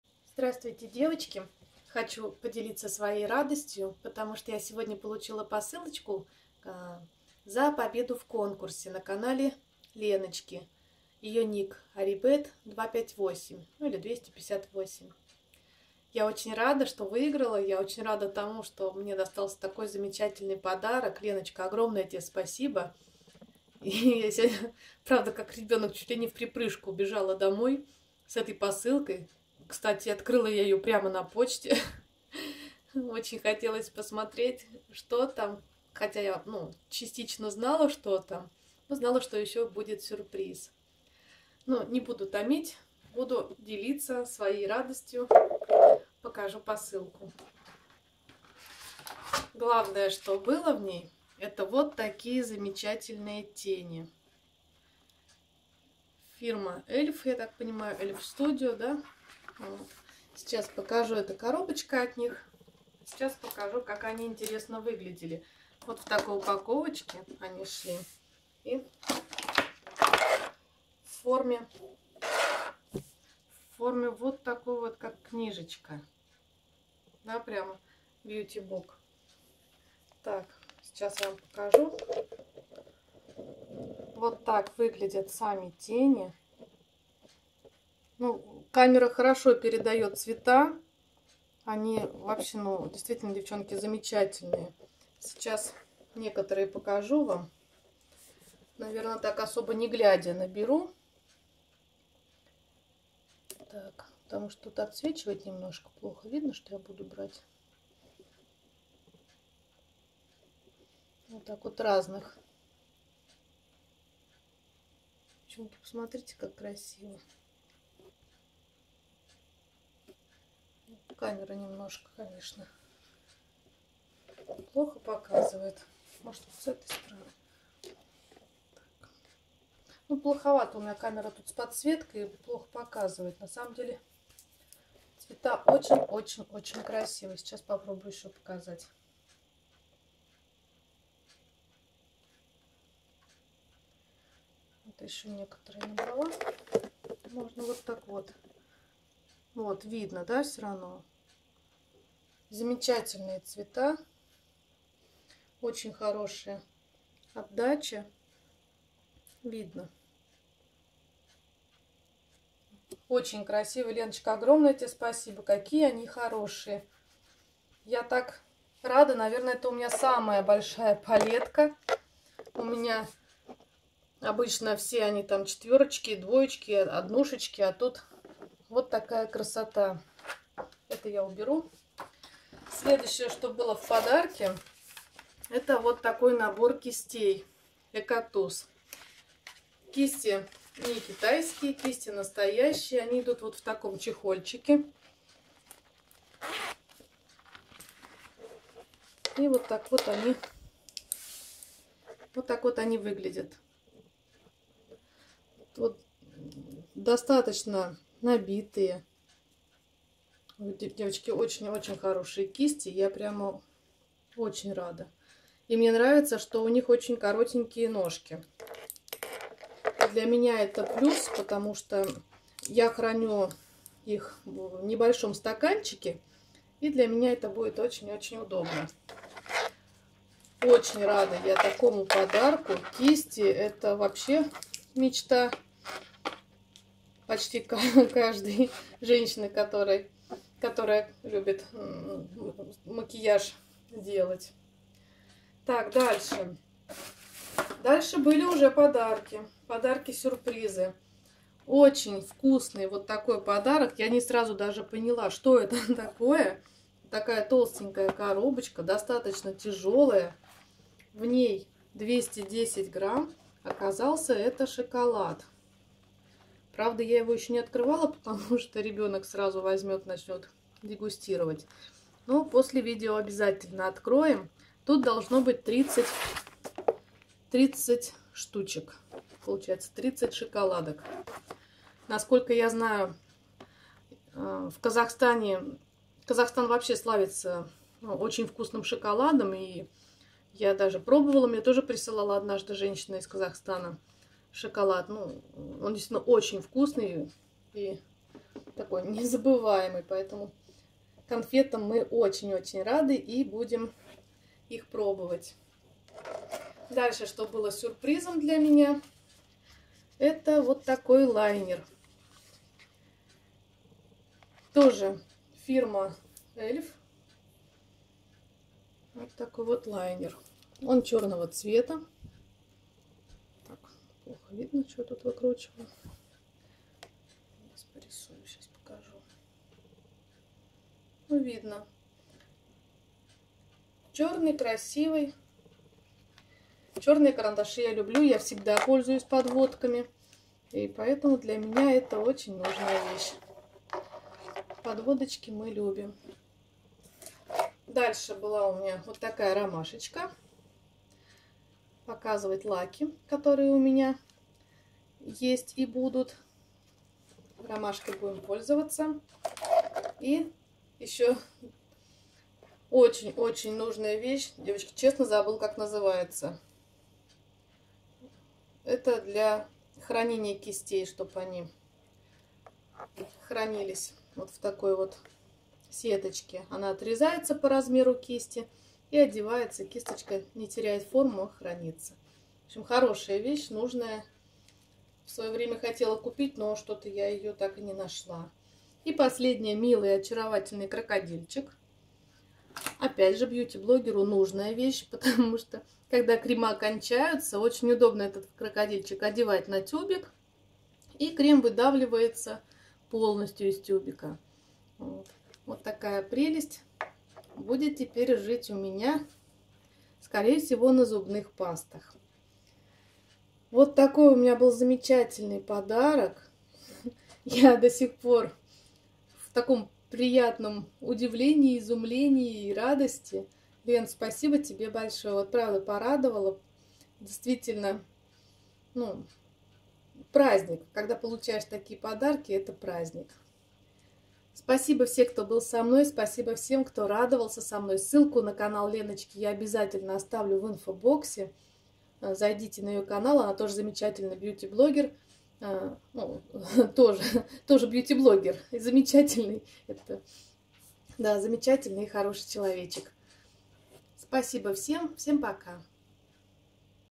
ПОДАРОК ЗА КОНКУРС ОТ ЛЕНОЧКИ (Aribet258)
Прошу прощение за то, что говорила часто слово ОЧЕНЬ. Видео немного сумбурное, но это от радости. Ссылка на канал Леночки, Aribet258